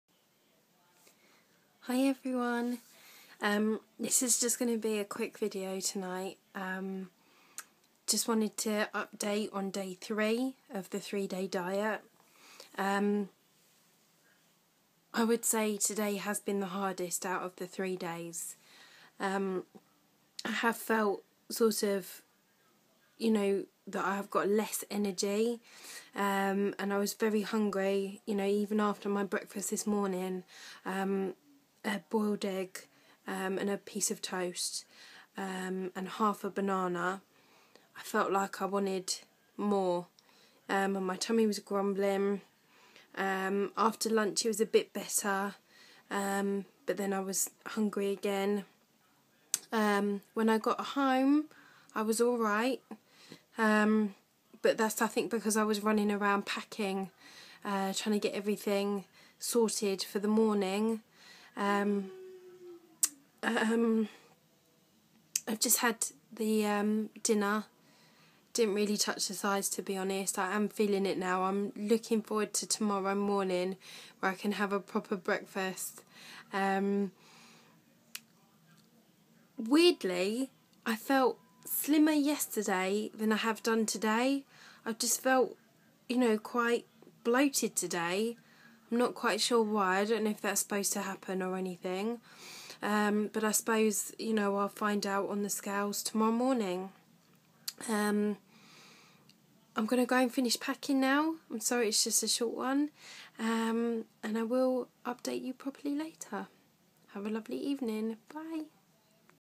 3 Day Diet - Day 3 Review
Just a short one tonight!! Very busy packing and feeling tired through lack of calories!!